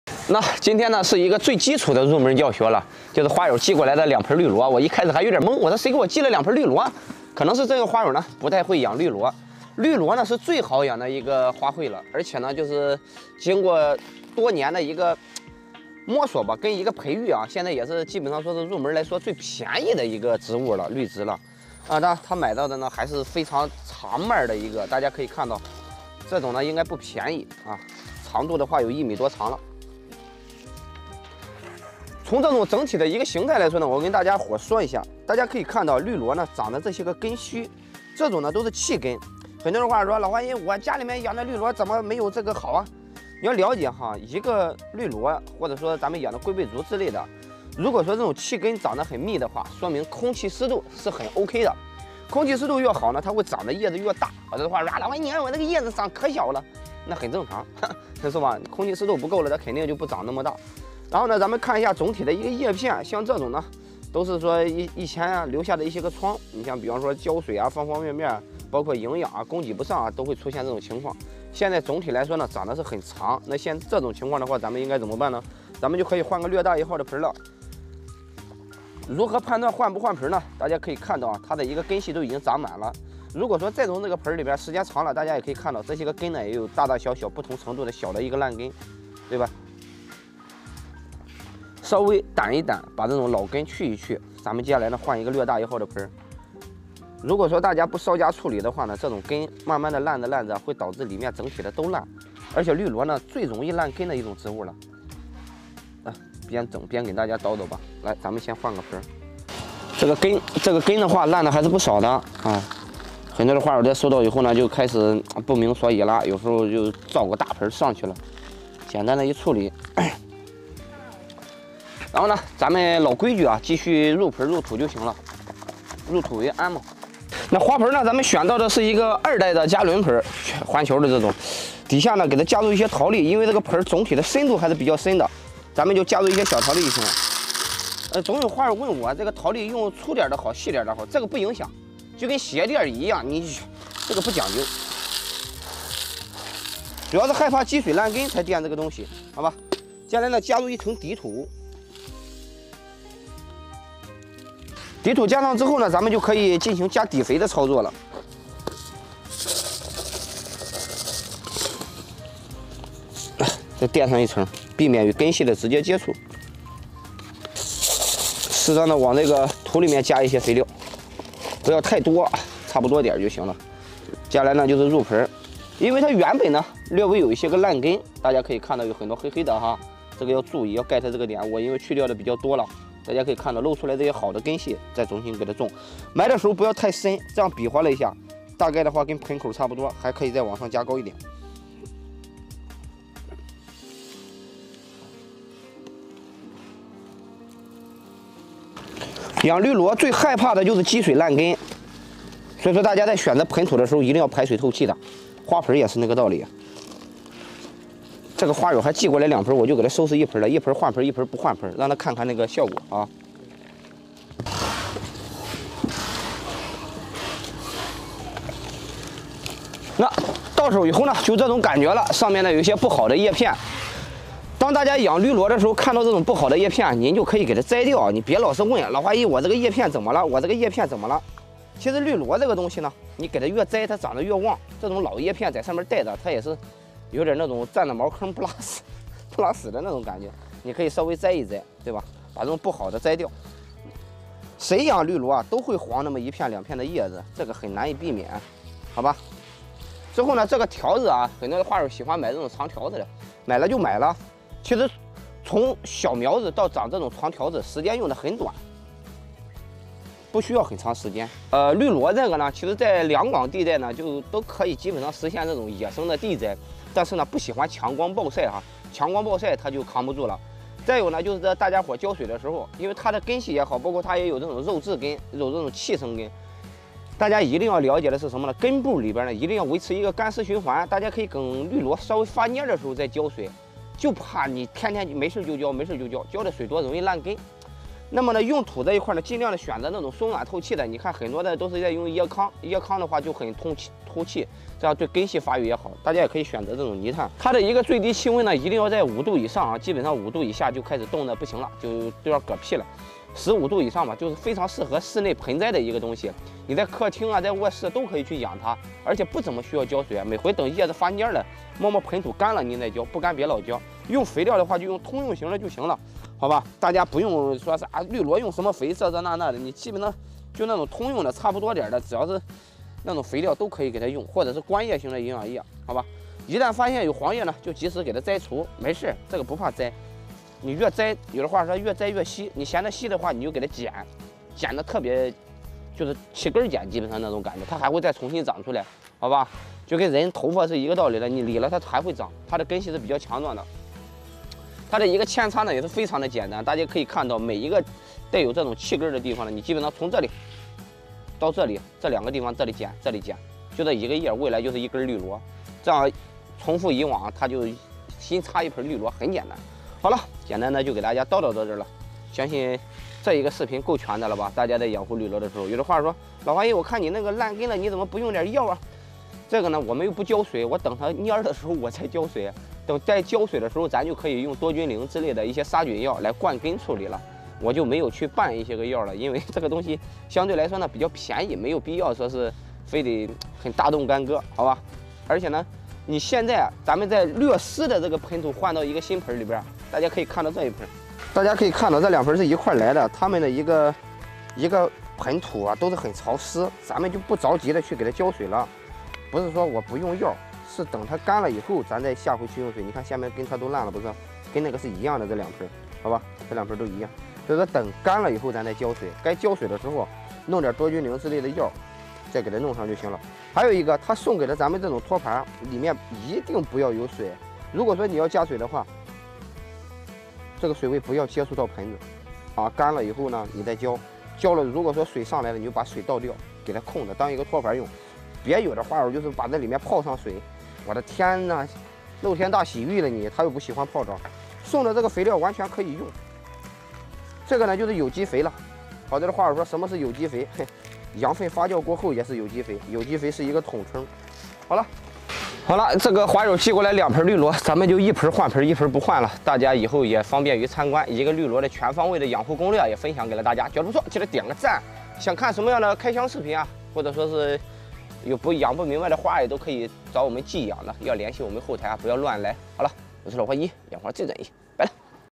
【寄养大绿萝】花友的绿萝根系竟然烂了这么多？全网最详细绿萝养护教程The most detailed video for Devil's ivy growing and care.
花友寄来两盆绿萝，全网最详细的绿萝养护教学来了！赶紧转发收藏。
A fan sent 2 pots of Devil's ivy. Laohuayi made this very detailed video for Devil's ivy growing and care.